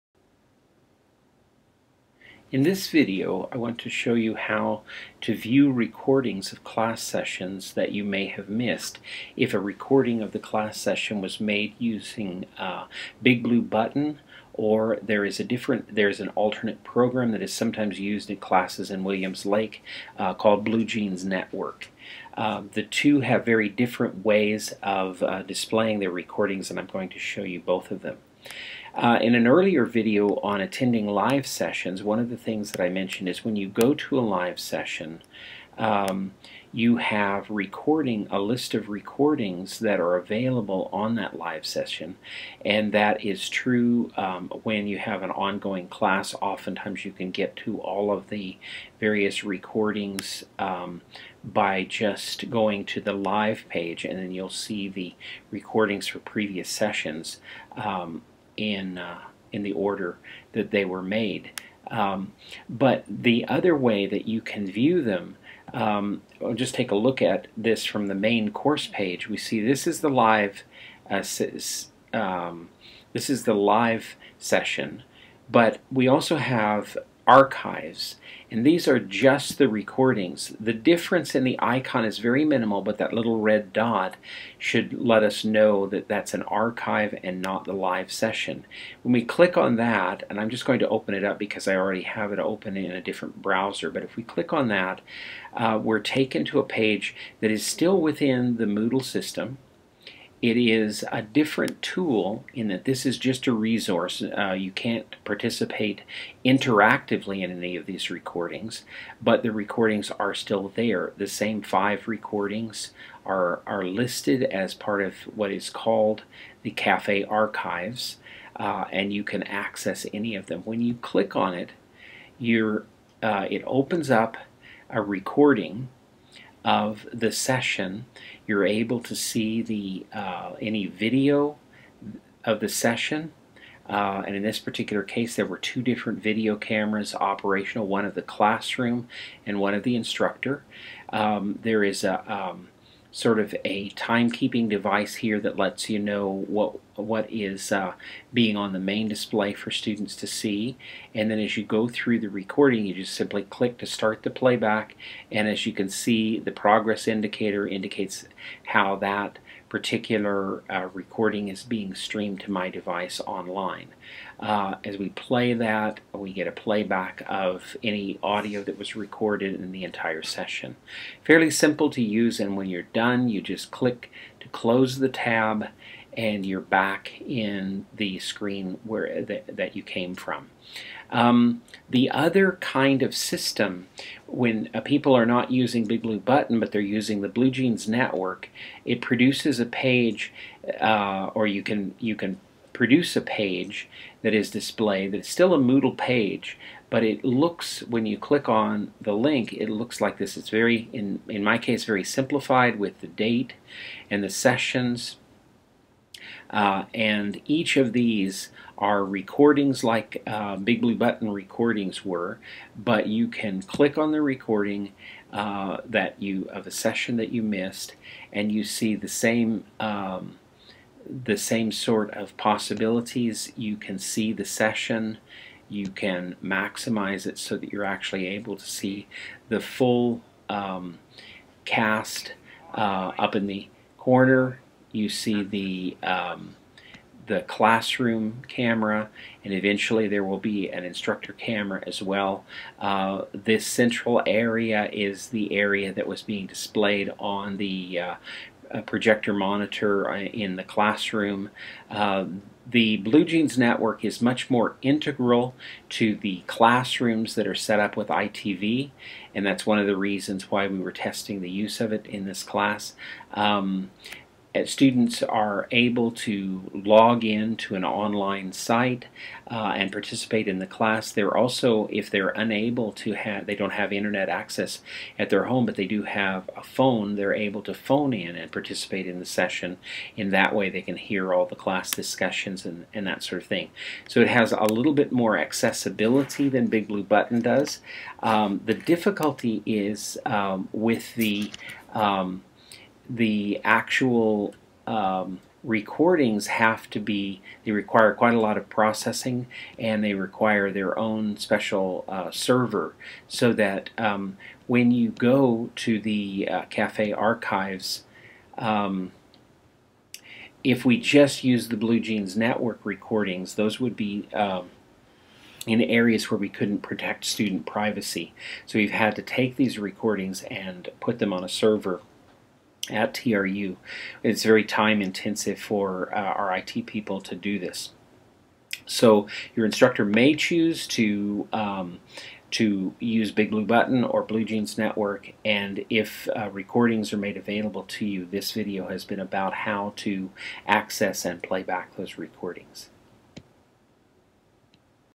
View Class Recordings
Students who have missed a class and who want to view the session recording need to know how to do this. This video shows how to view recordings made with Big Blue Button or using the Blue Jeans Network.